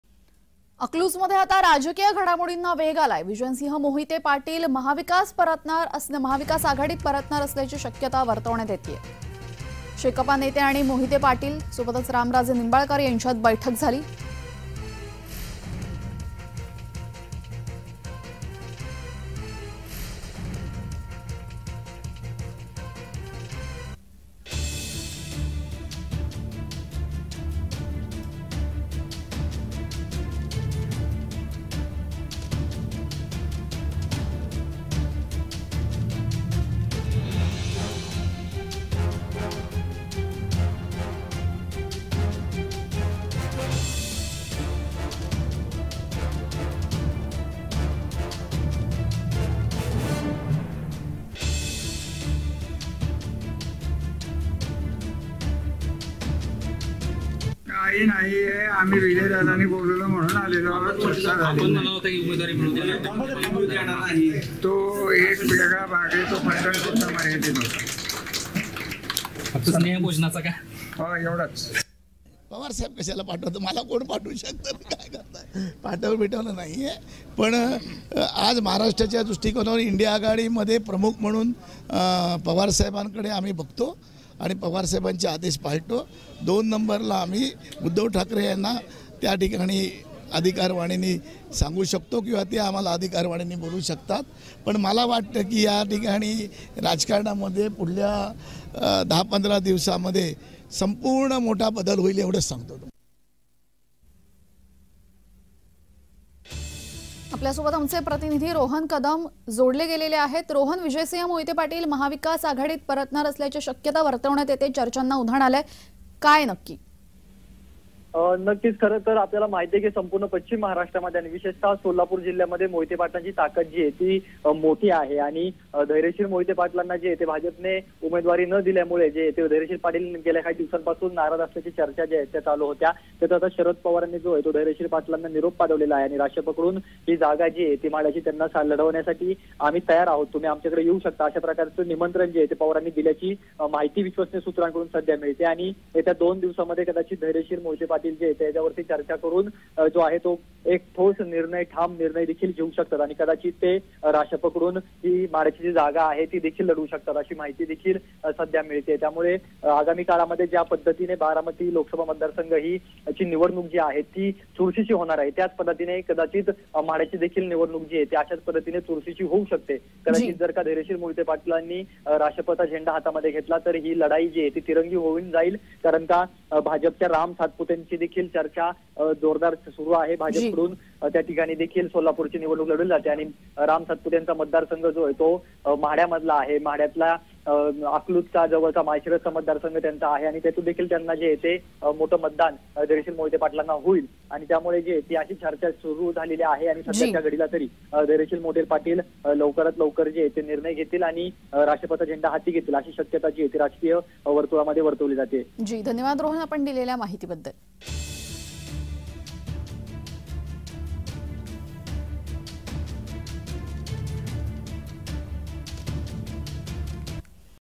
VijaySingh Mohite Patil | विजयसिंह मोहिते पाटील मविआत परतणार ? | Marathi News
अकलूजमध्ये राजकीय घडामोडींना वेग
विजयसिंह मोहिते पाटील मविआत परतणार ?
शेकाप नेते, मोहिते पाटील आणि रामराजे निंबाळकर यांच्यात बैठक